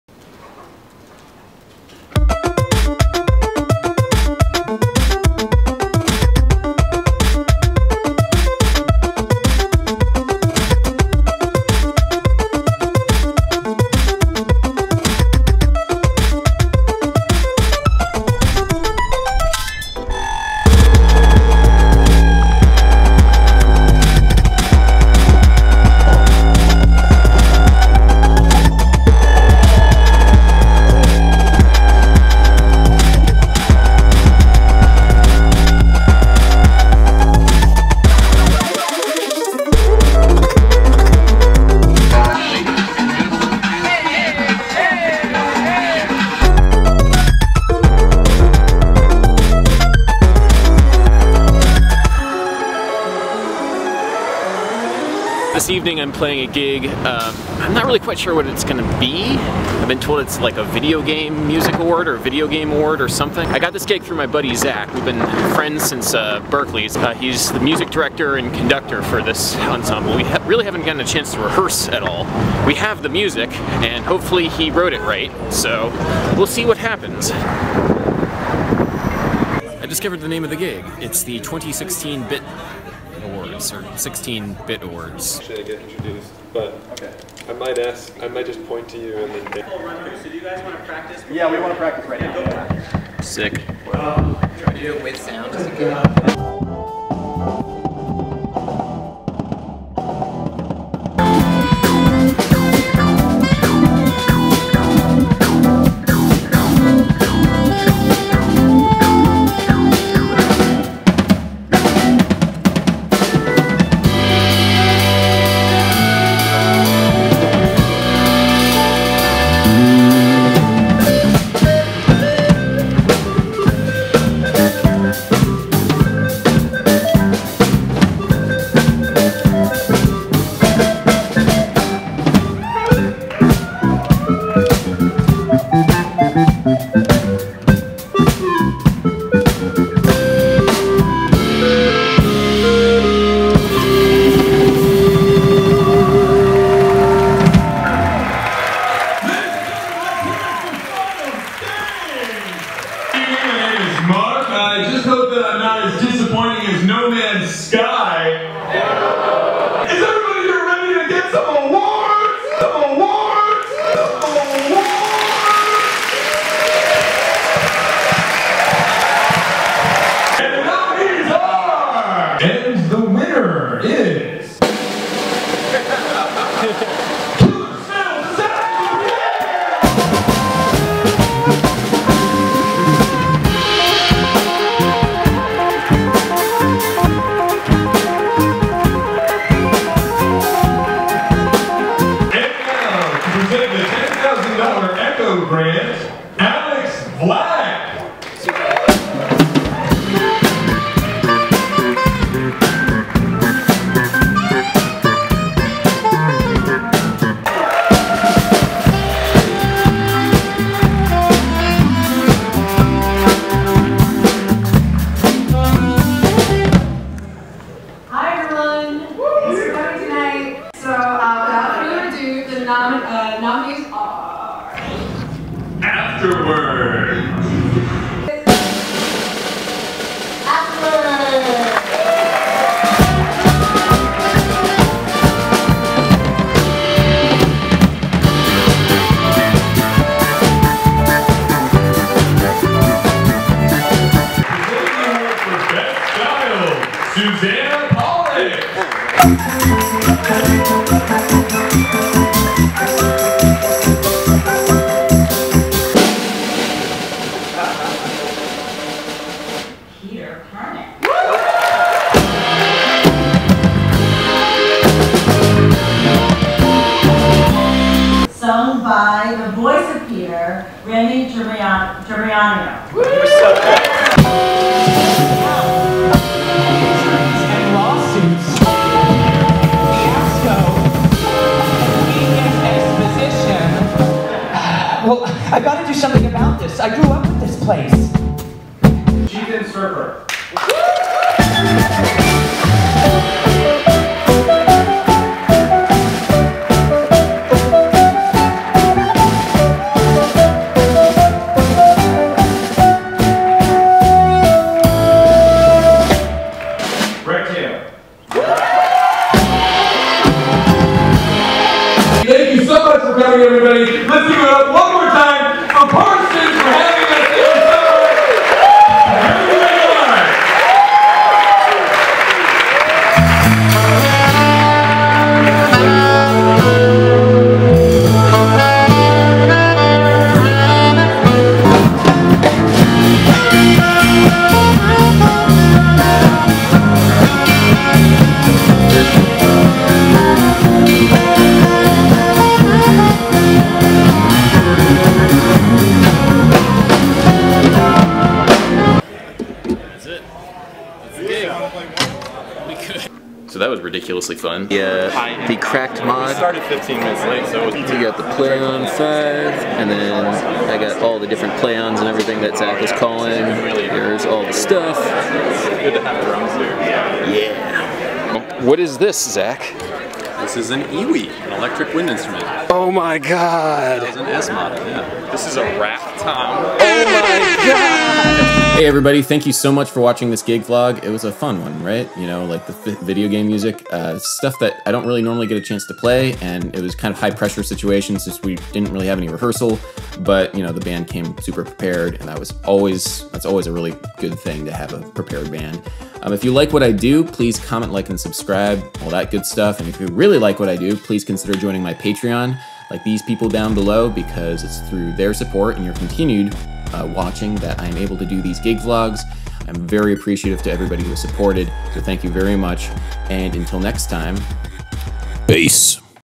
'16 Bit Video Game Awards | Gig Vlog #5
The '16 Bit Video Game Awards were presented by Playcrafting, check them out.

Zac Zinger - EWI, Sax, MD
Luke Markham - Drums
Takafumi Suenaga - Keys
Me - bass

Peace,

Adam